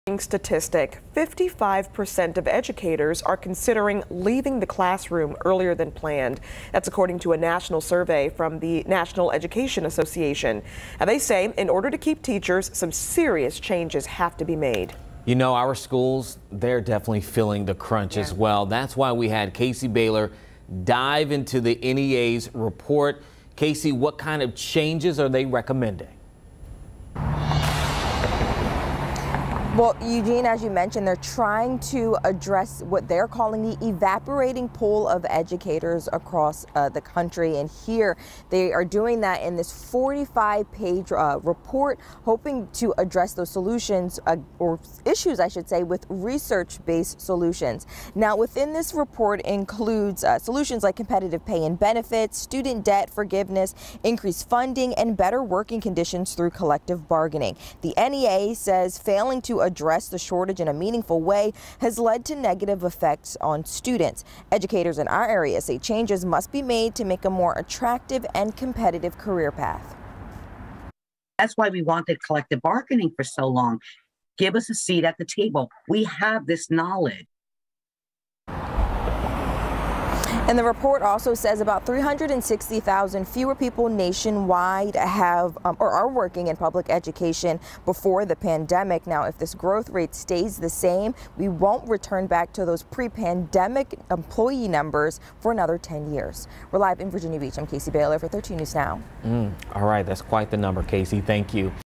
Sobering statistics: Educators consider leaving the classrooms in large amounts
Teachers say that major systemic changes must be made in order to truly help them not only survive, but thrive.